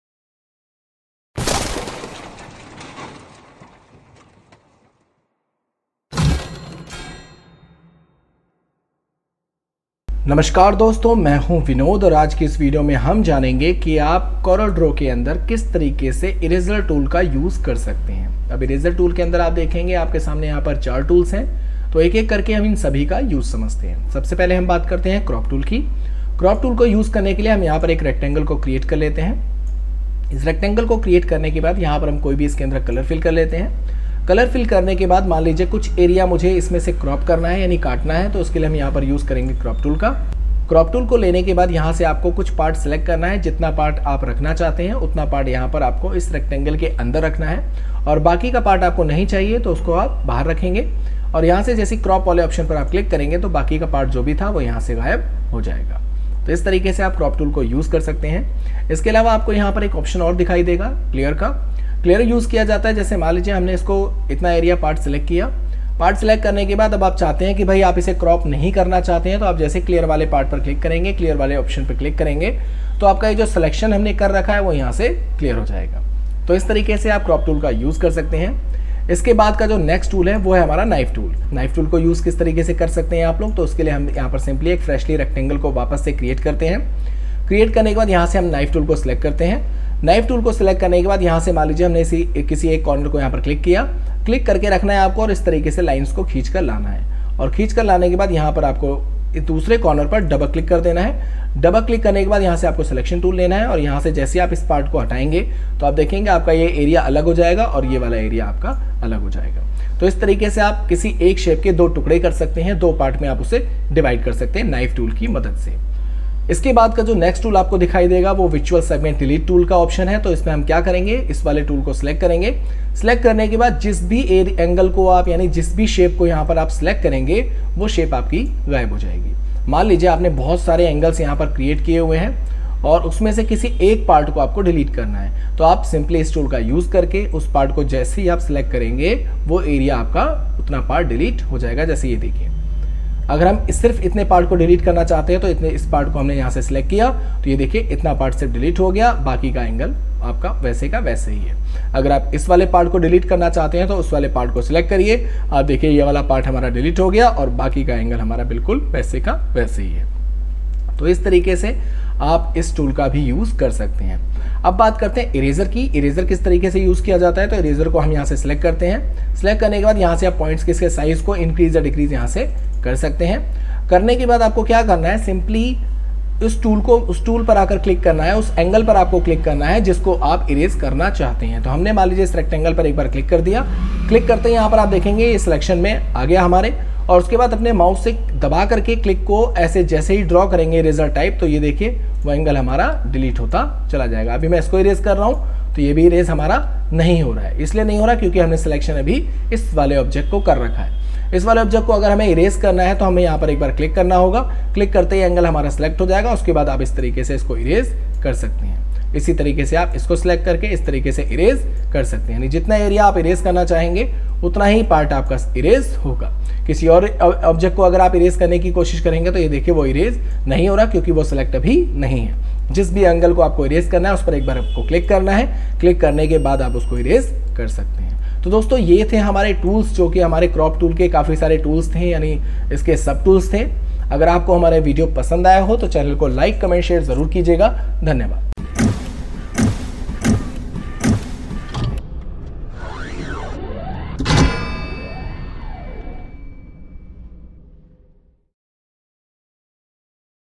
Crop tools in coreldraw 2021 - Coreldraw crop tool tutorial hindi
in this video I will show you how to use crop tool Corel draw 2021 hindi

Es video mai aapko corel ke kuch tool ka basic use btaya gya hai
1.How to Learn Tool Crop, Knife and Erase using Corel draw
2. How to Crop Image in CorelDraw
3.How to Use the Crop Tool in Corel Draw